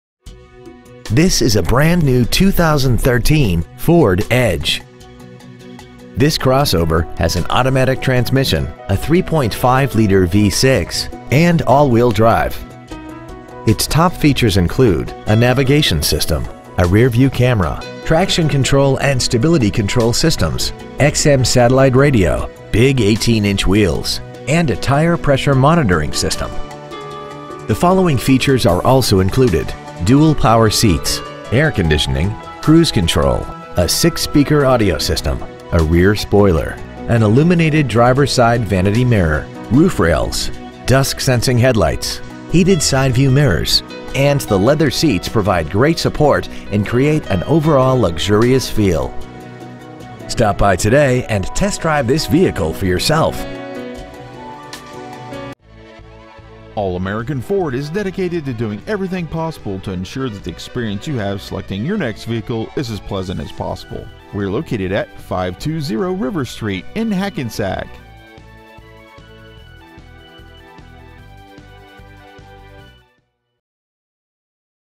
2013 FORD EDGE Hackensack NJ
Year : 2013
 Make : FORD
 Model : EDGE
 Engine : 3.5L V6 24V MPFI DOHC
 Trans . : 6-SPEED AUTOMATIC
 Exterior : INGOT SILVER METALLIC
 Miles : 16
 Interior : CHARCOAL BLACK
 Stock : 13T1838